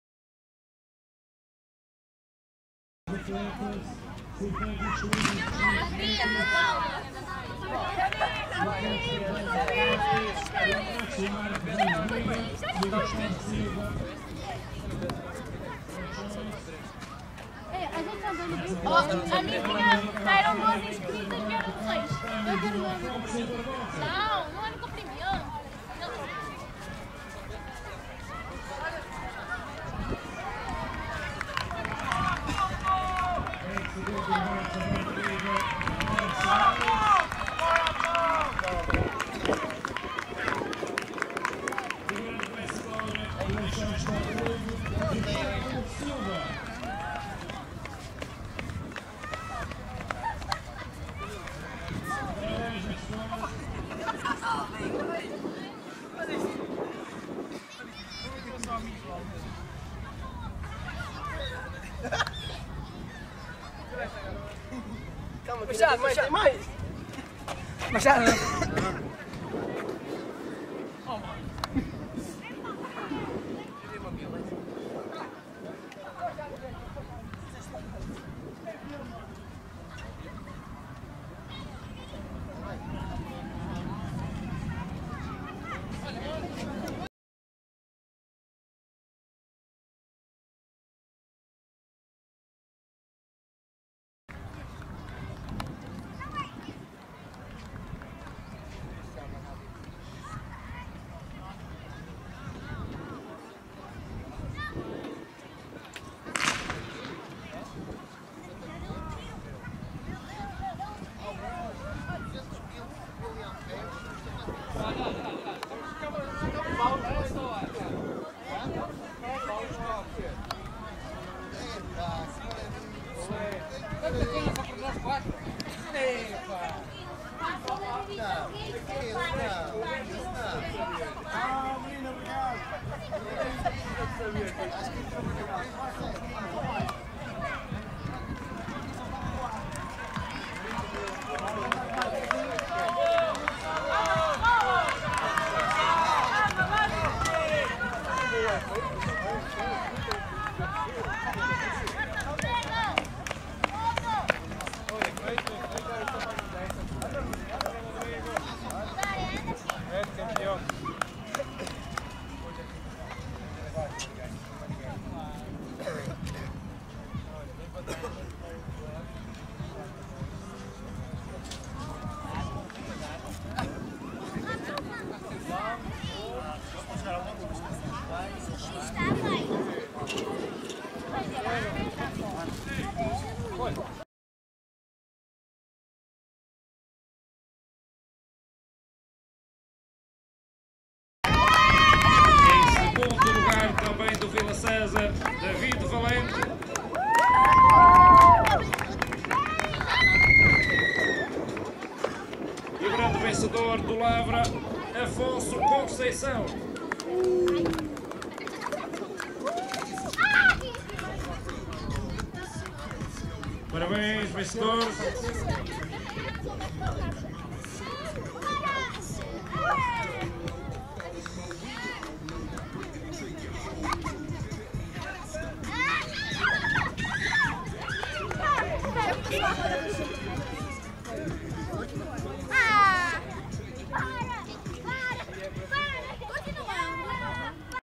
Meeting Atletismo Matosinhos CUP 2023: 300m M18
Meeting Atletismo Matosinhos CUP 2023: 300 Metros Sub 18 Masculinos. Prova disputada no Estádio Municipal de Leça da Palmeira, na tarde de 5ª feira, feriado nacional, dia 5 de outubro de 2023.